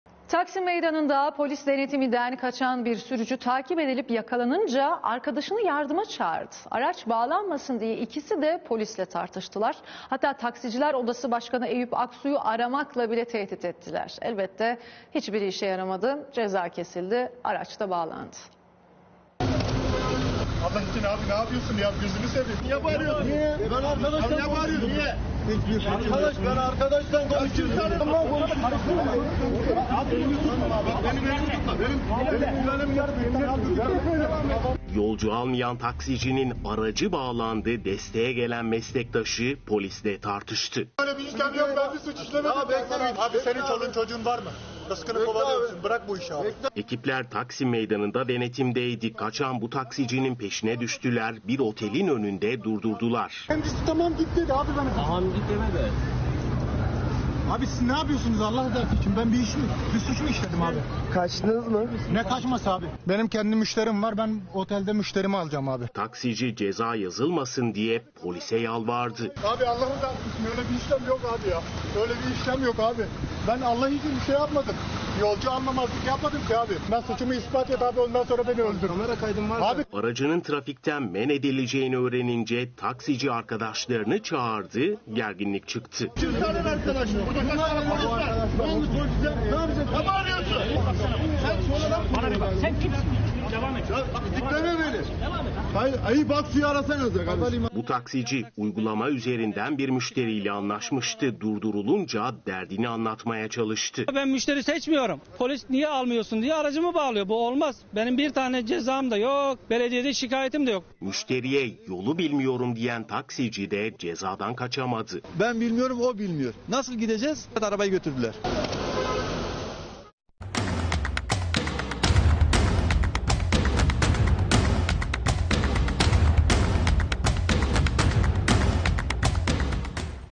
Taksiciler polisle tartıştı
TAKSİM MEYDANI'NDA POLİS DENETİMİNDEN KAÇAN BİR SÜRÜCÜ, TAKİP EDİLİP YAKALANINCA ARKADAŞINI YARDIMA ÇAĞIRDI. ARAÇ BAĞLANMASIN DİYE İKİSİ DE POLİSLE TARTIŞTILAR, HATTA TAKSİCİLER ODASI BAŞKANI EYÜP AKSU'YU ARAMAKLA BİLE TEHDİT ETTİLER.  ELBETTE HİÇBİRİ İŞE YARAMADI, CEZA KESİLDİ, ARAÇ BAĞLANDI.